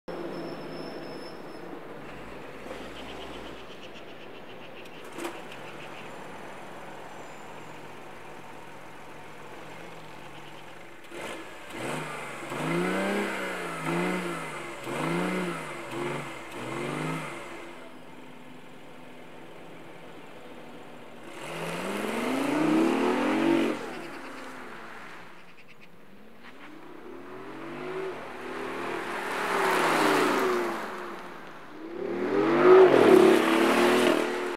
BMW I8 2016 - START UP, SOUND, REVS & ACCELERATIONS
I hope you've enjoyed the video and don't forget to subscribes to my channel for more videos about cars and motorbikes!